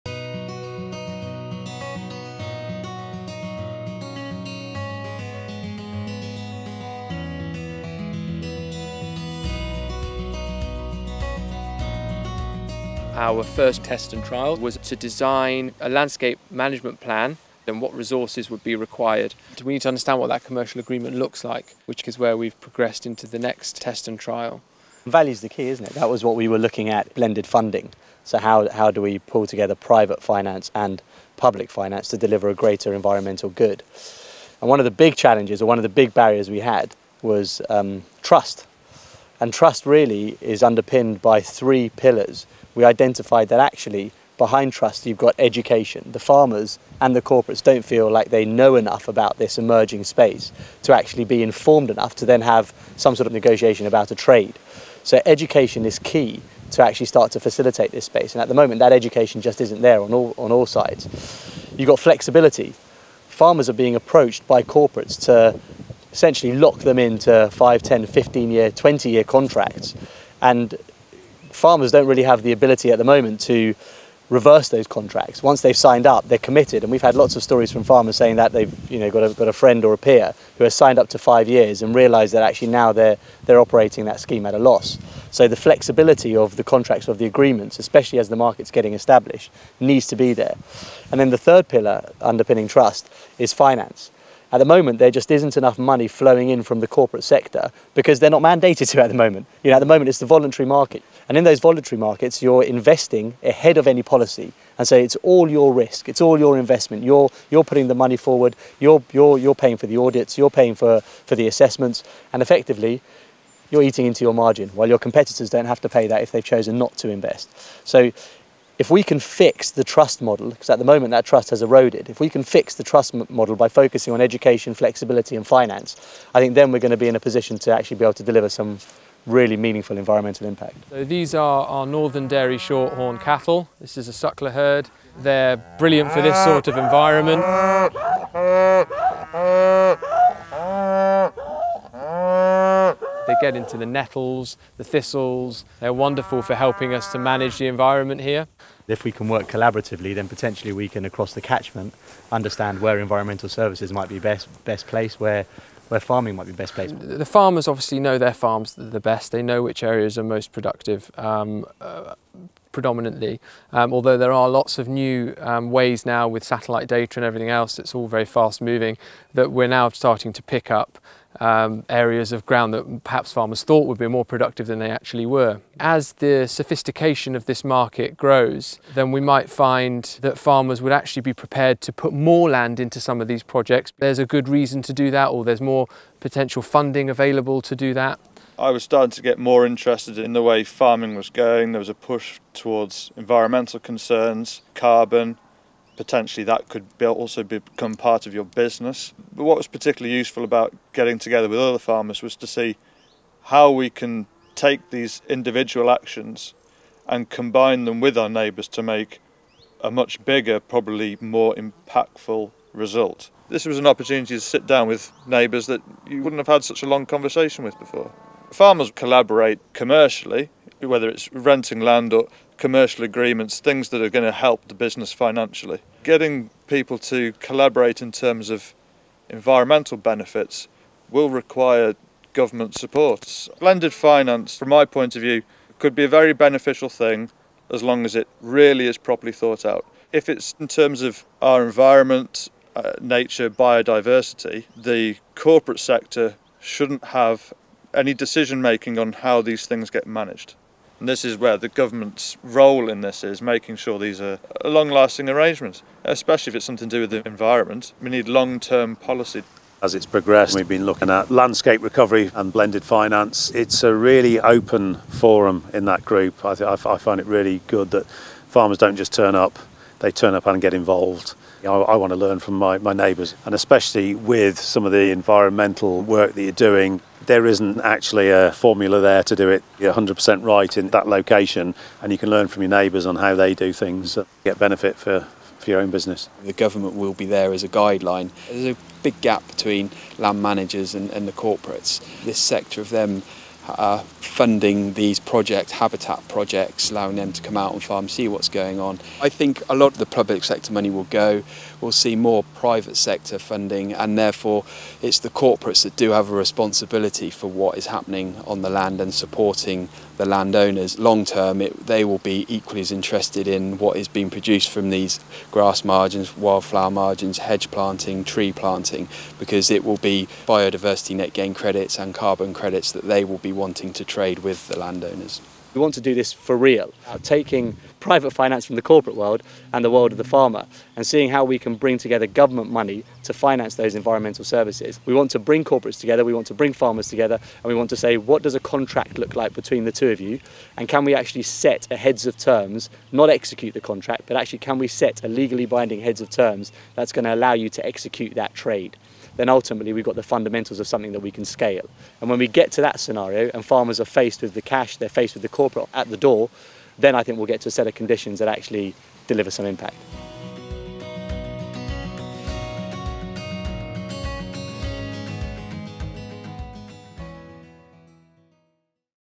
Aqualate Mere Test & Trial update part 2
Farmers from the Aqualate Mere Catchment share their experiences of taking part in Tests and Trials. This test focused on building commercial agreements between private organisations and local farming communities.

This is the second of a 2-part series on the work of Aqualate Castle Holdings in Tests and Trials. This test followed on from the findings of a first test that focused on creating a land management plan and worked to set priorities for the Catchment, incorporating biodiversity, water quality and carbon sequestration. This was explored in the first video.